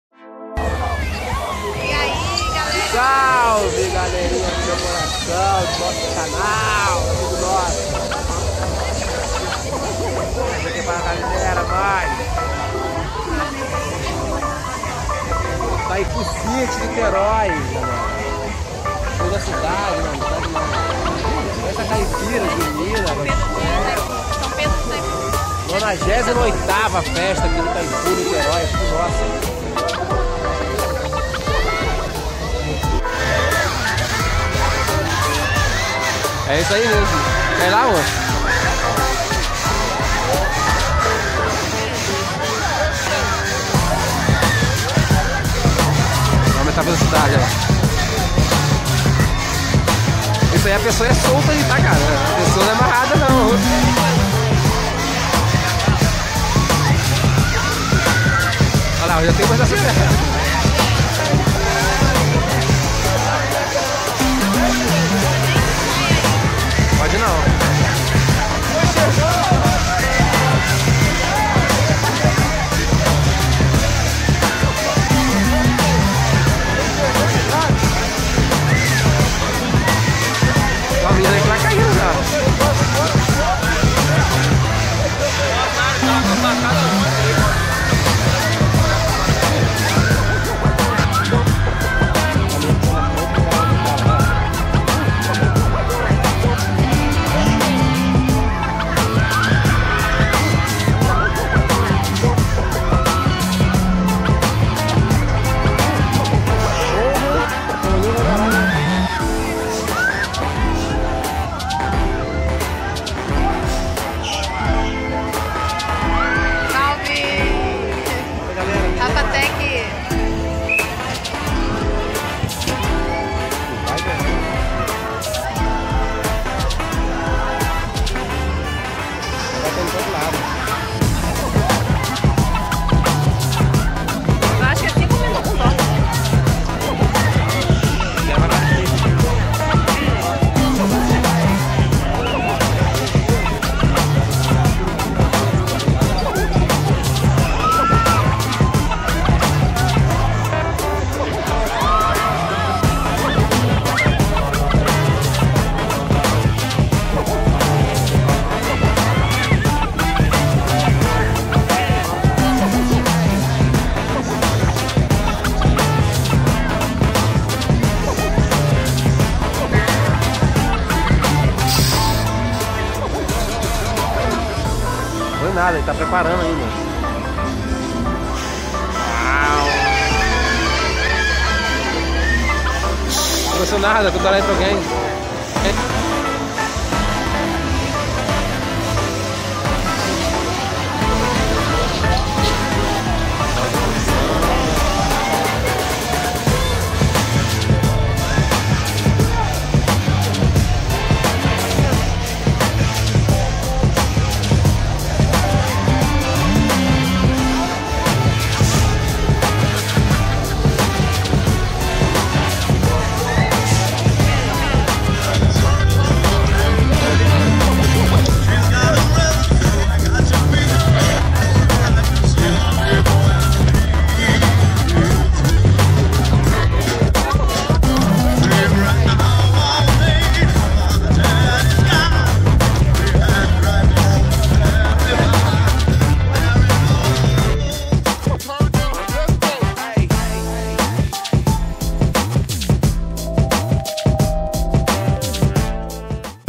Fomos na Festa de São Pedro de Itaipu Niterói-RJ 2018
98º Festa de São Pedro em Itaipu Itaipu Niterói-RJ, pessoas brincando no parque.